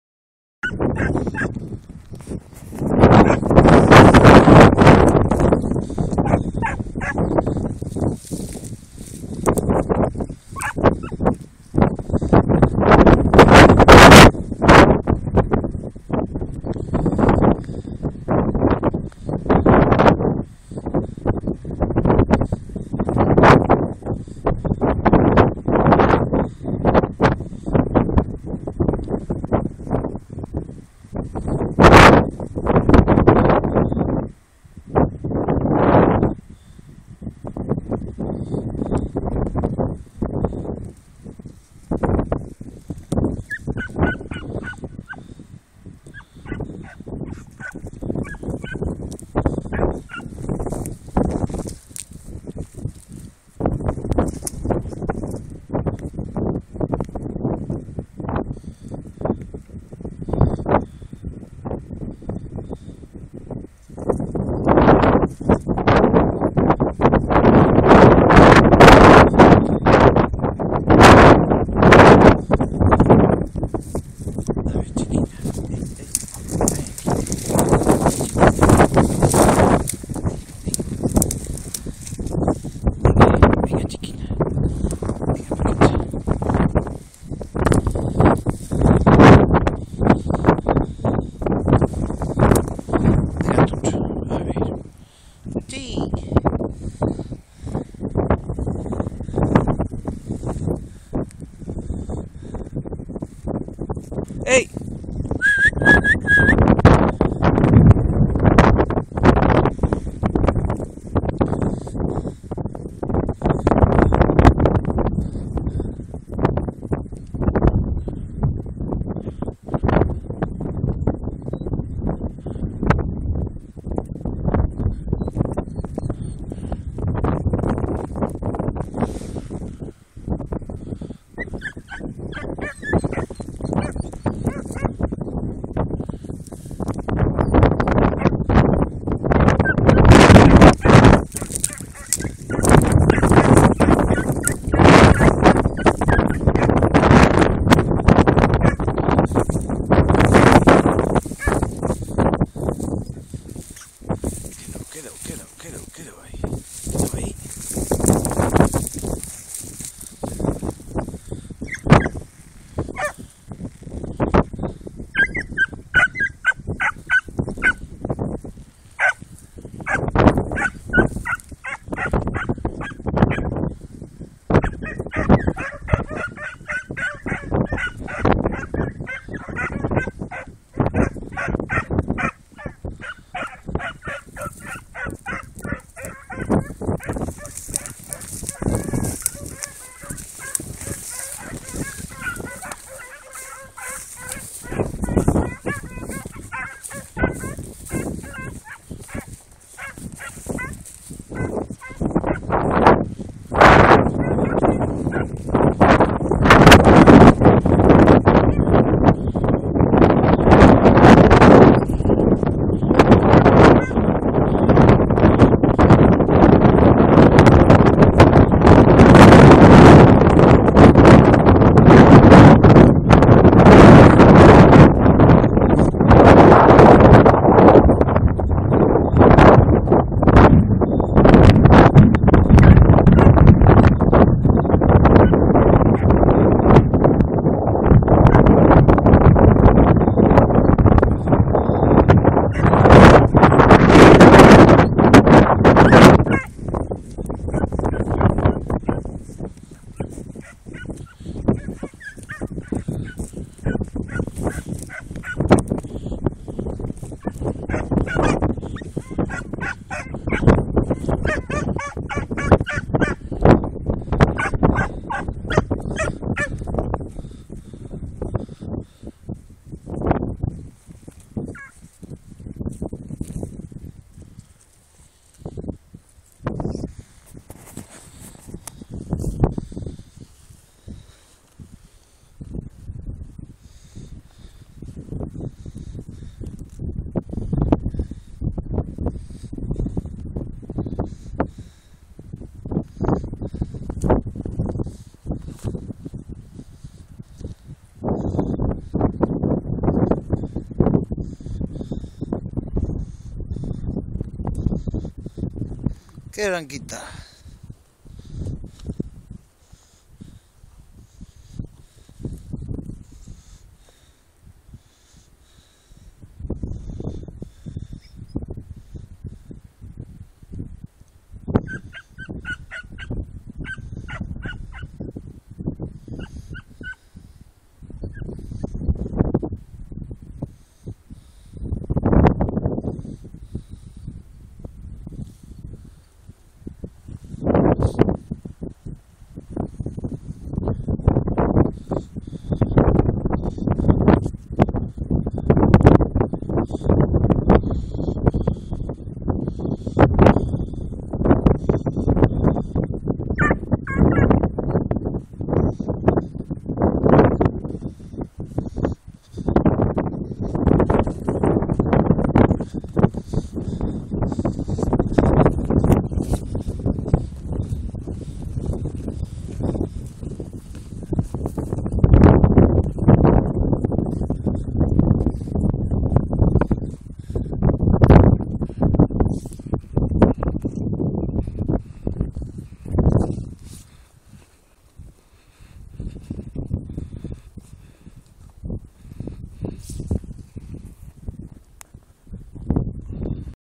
PBGV. Aprendiendo a seguir rastros de conejo
Cachorros de PBGV. Cuatro meses y medio.